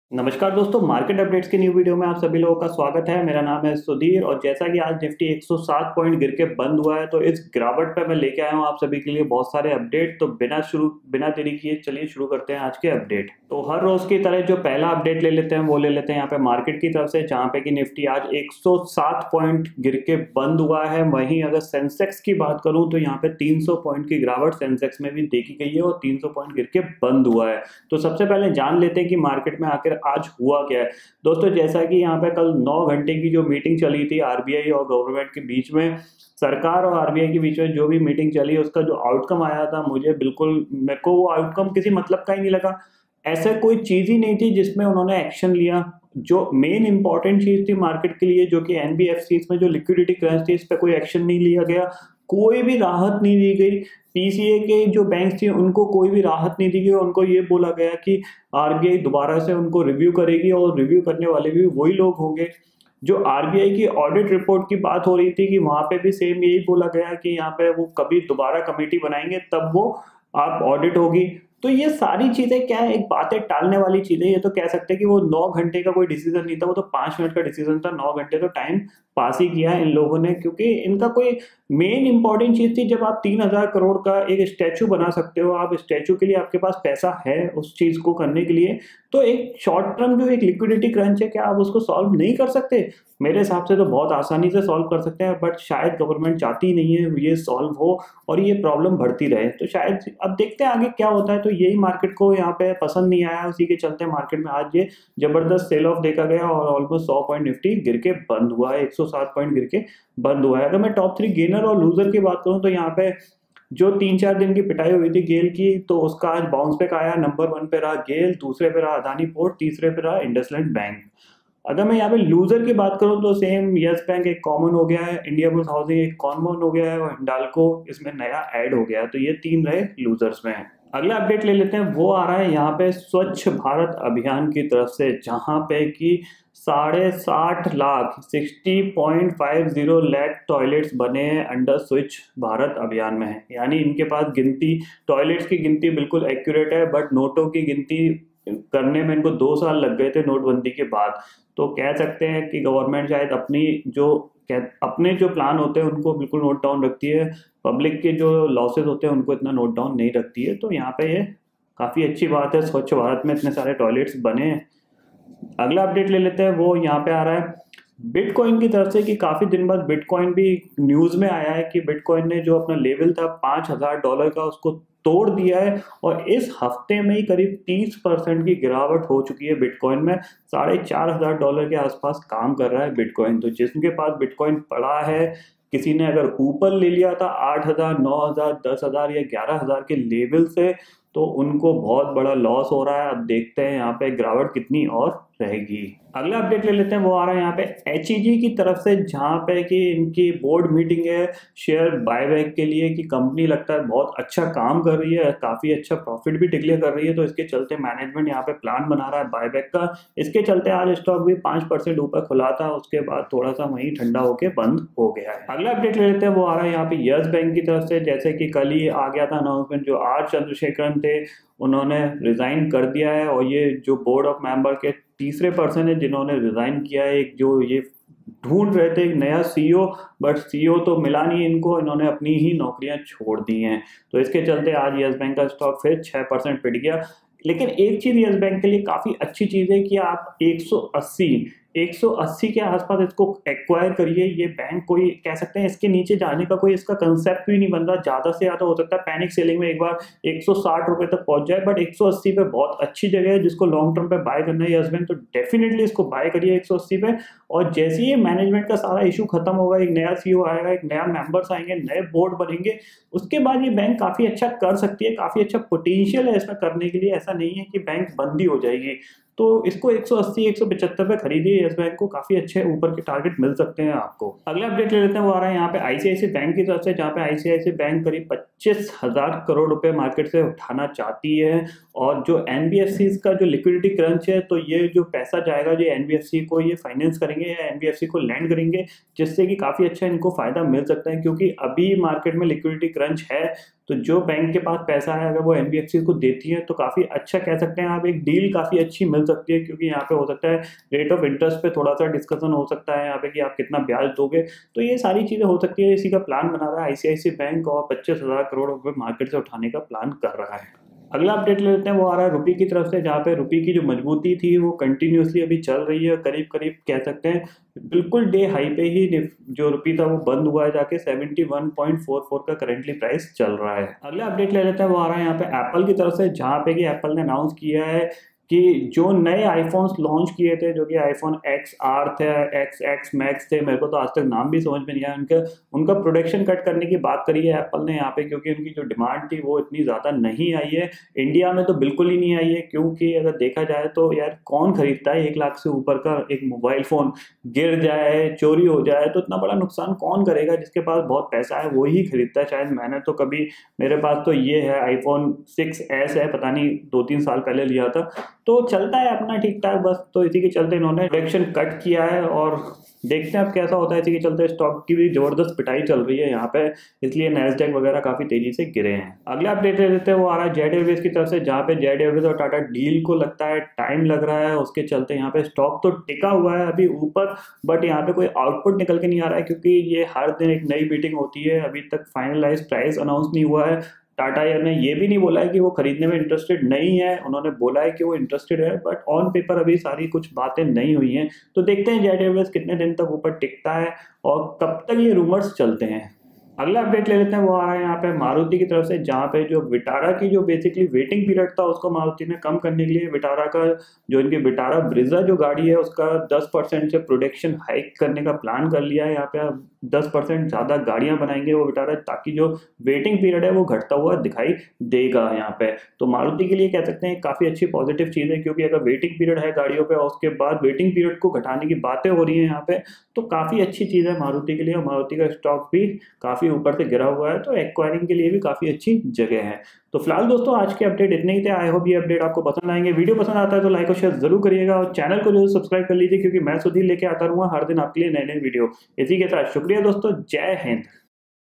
Market Update #48 Nifty Fall 107 Point, Bitcoin breaches $5,000, hits fresh 13-month low
Namaskar dosto, aaj ke market update me Nifty fall 107 point, Nearly 60.50 lakh household toilets built under Swachh Bharat,
Bitcoin breaches $5,000, hits fresh 13-month low, HEG gains 5% as board to consider share buyback proposal, ICICI Bank may raise Rs 25,000 crore to lend amid NBFC liquidity crunch, Vitara Brezza waiting period to come down as Maruti ramps up production..

wealthsky !!! keep watching !!! keep supporting !!! Thank You